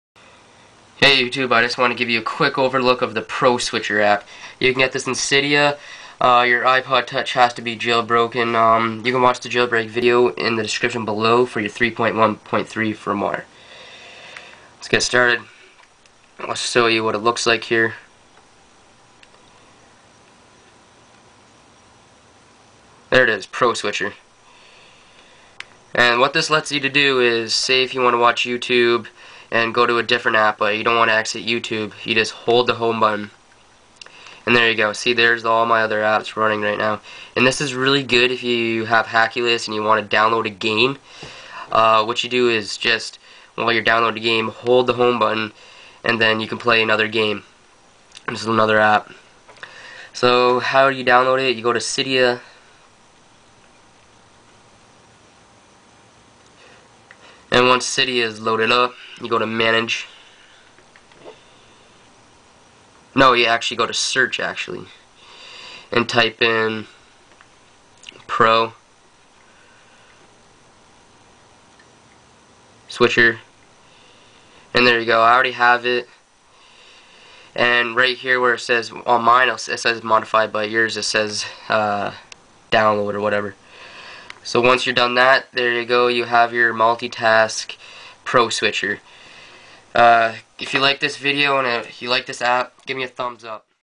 ipod touch pro switcher app - multi task with iphone and ipod touch
this app lets you multi task with opening other apps at one time